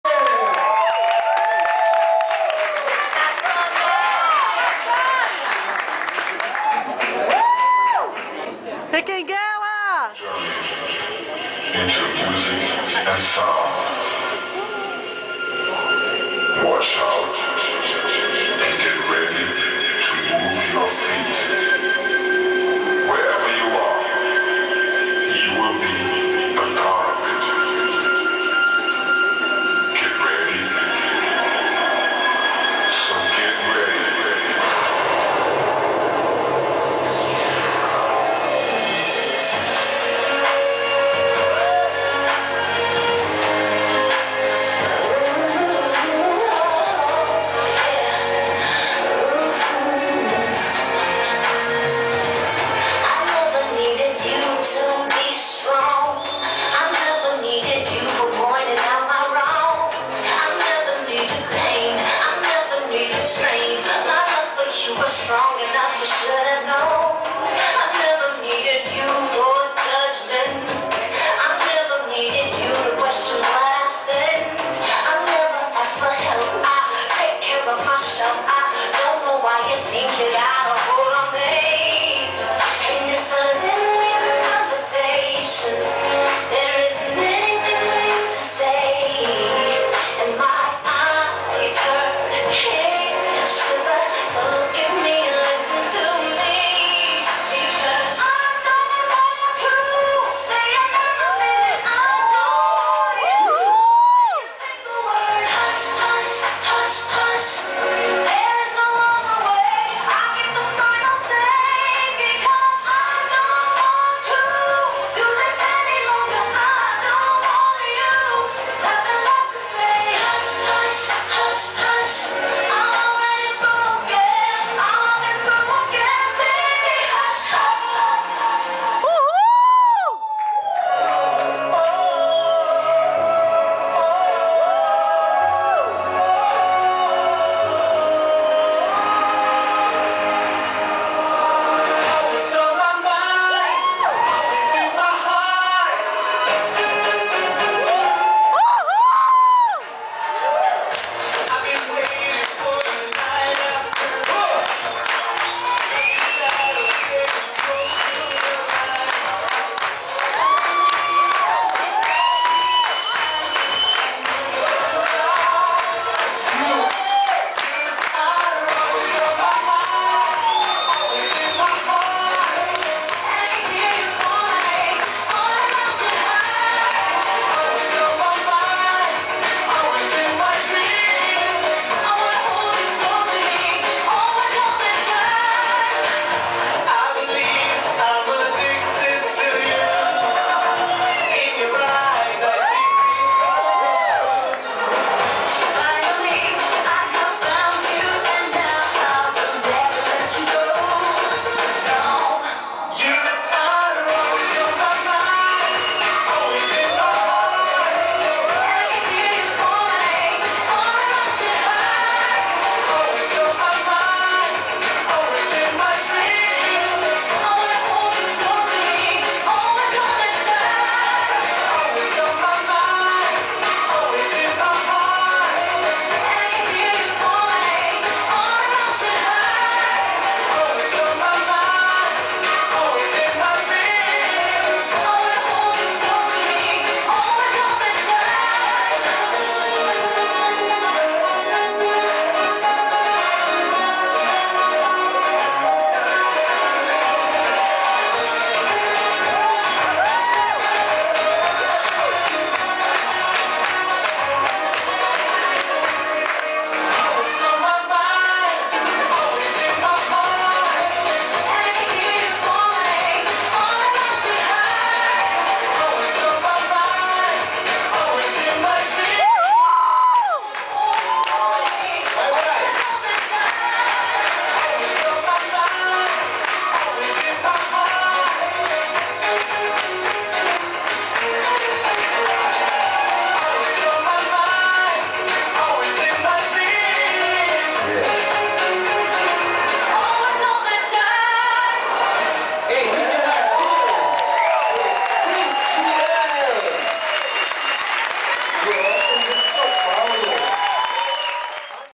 3° Brazil Zouk Congress: Henrique & Cris Minato
Apresentação dos professores Henrique Martins e Cris Minato na academia Interacto - Sp no 3° Brazil Zouk Congress